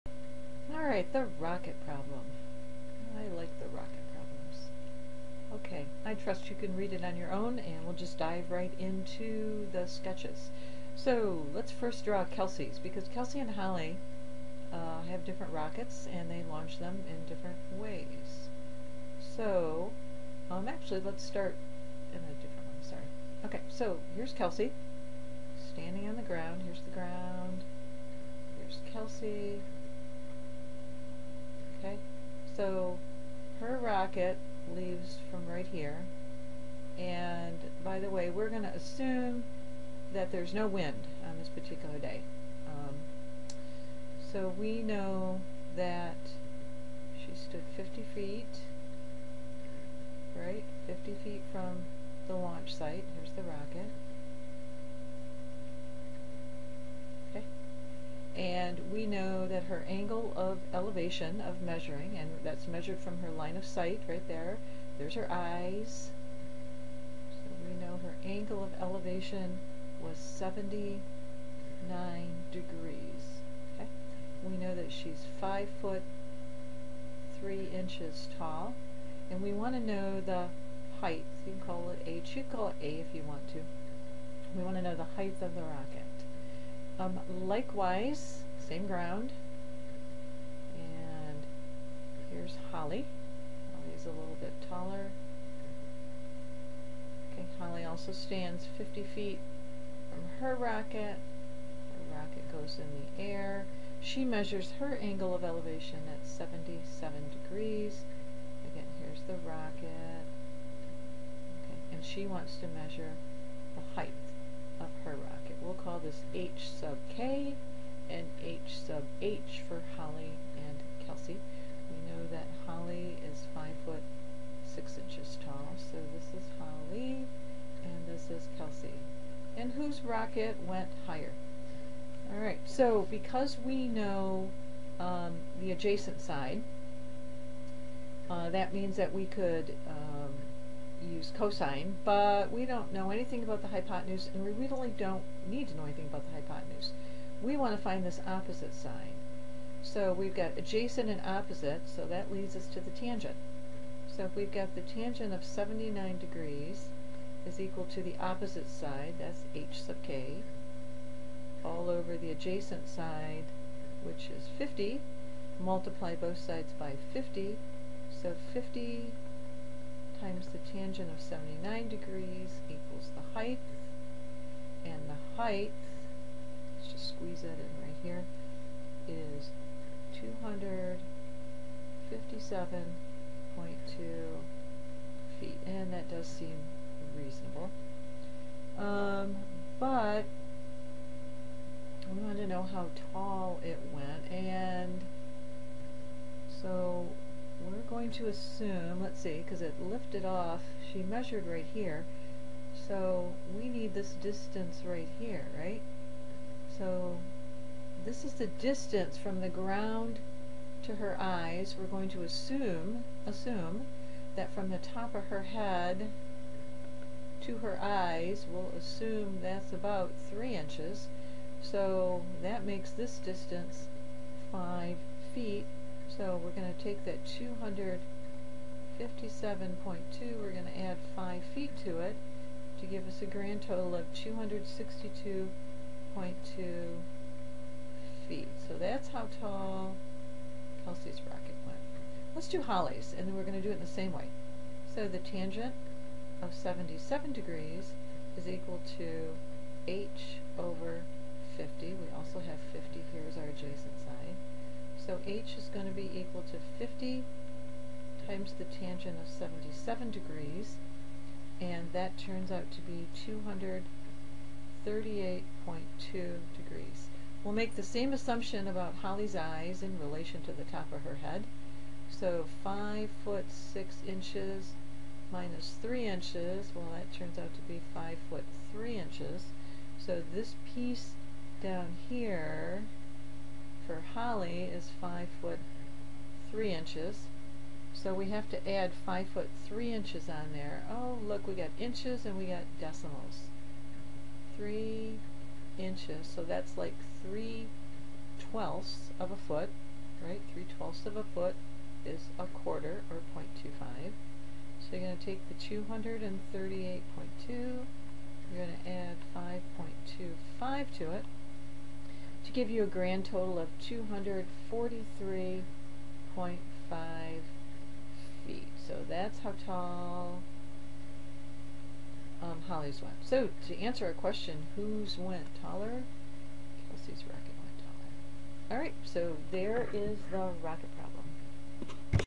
3.3 Application of Right Triangle
An application problem of right triangles; the rocket problem.